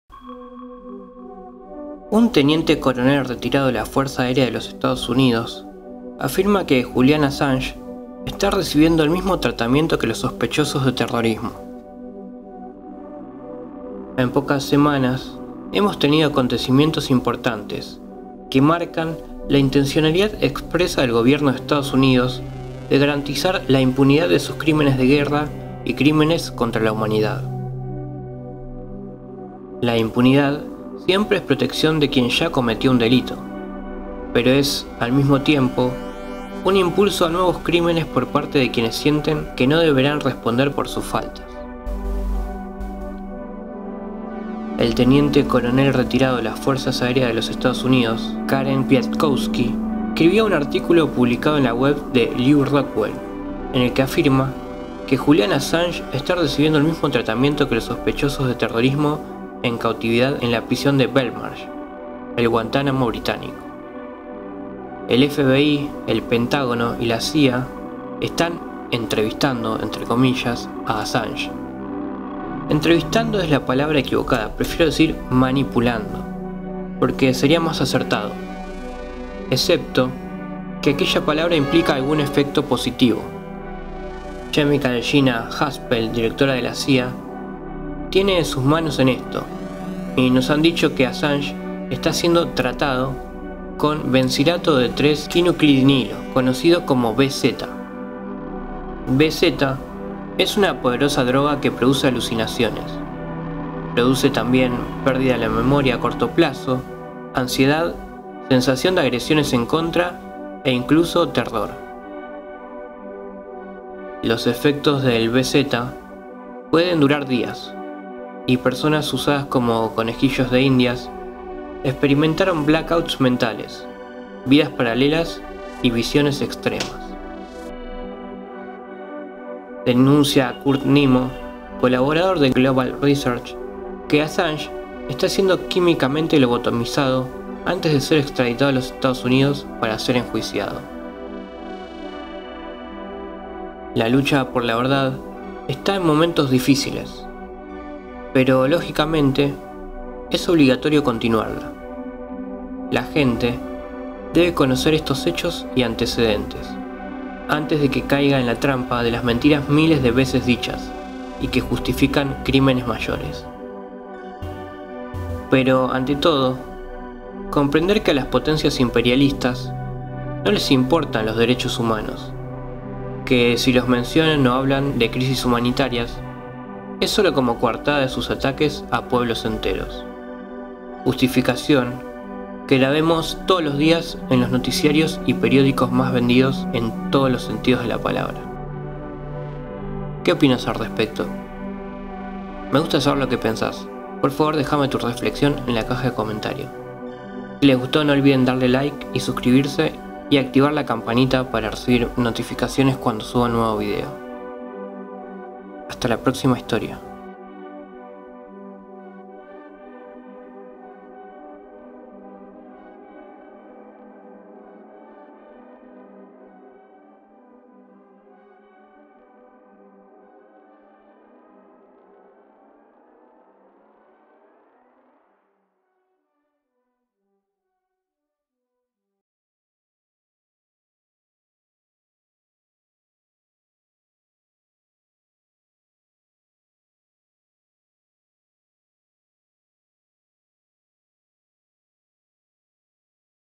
Assange censurado
A veces los peores terrores no son paranormales. En esta ocasión indagaremos la reciente denuncia de una ex coronel de la fuerza aérea de los EE.UU.

Autor Cancillería del Ecuador
Author Michael Vadon
Author David B. Gleason

Author David G Silvers.

Public Domain

Autor Photo by Petty Officer 1st class Shane T. McCoy, U.S. Navy